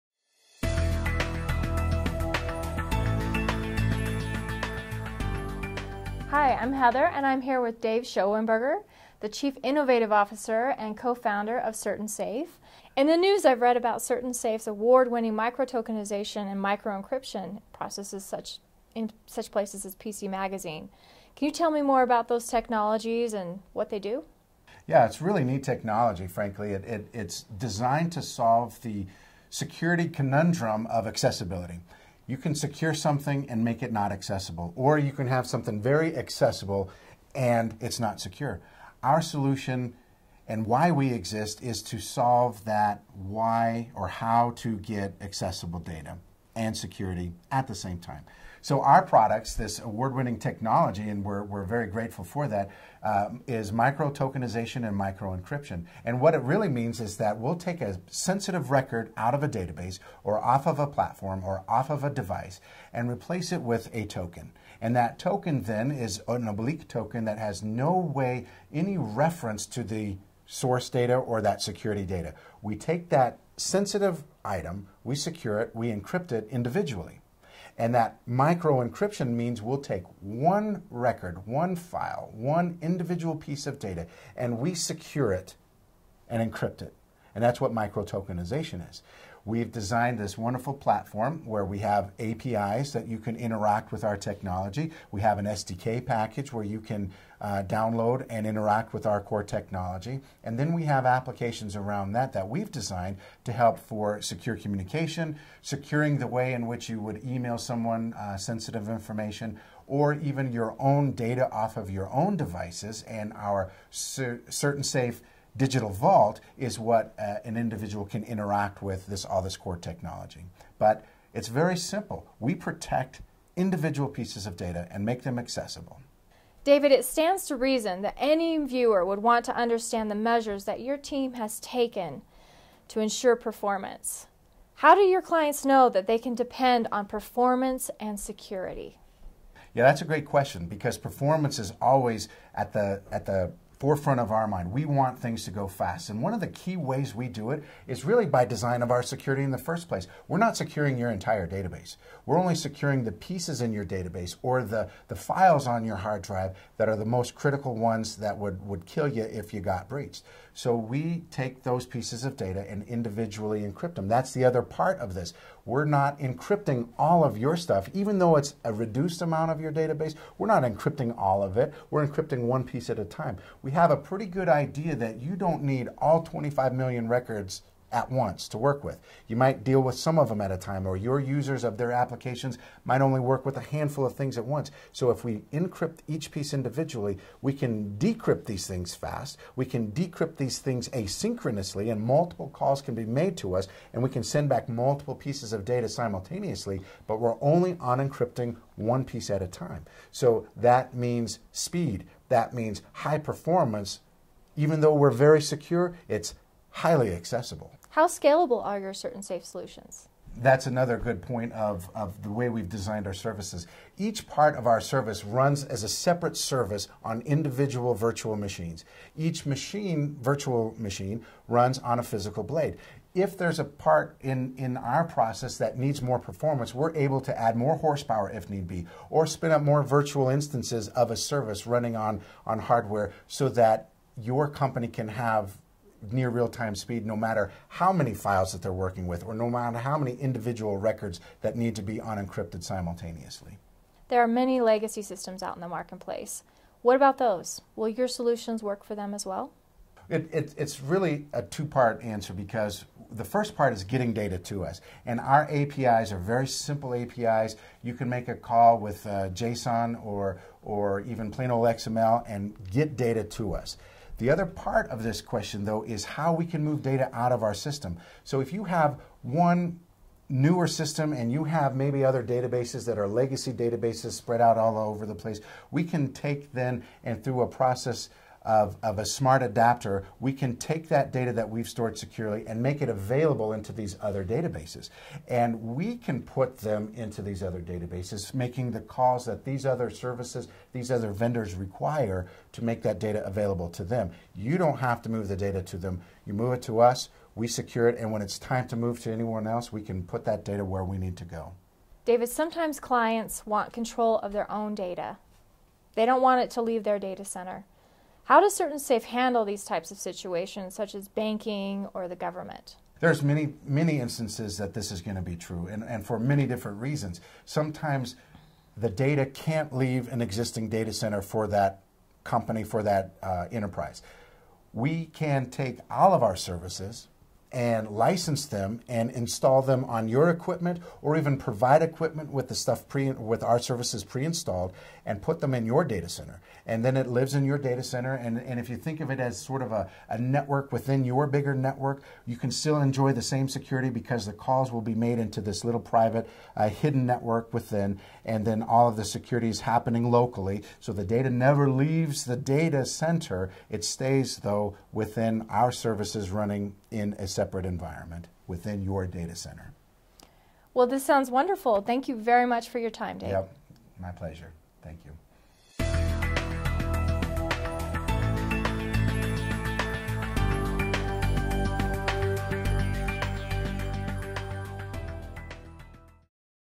The CertainSafe Difference 815
A visit with David Schoenberger, CertainSafe's CIO and co-creator of this incredibile technology, describing "The CertainSafe Differnece".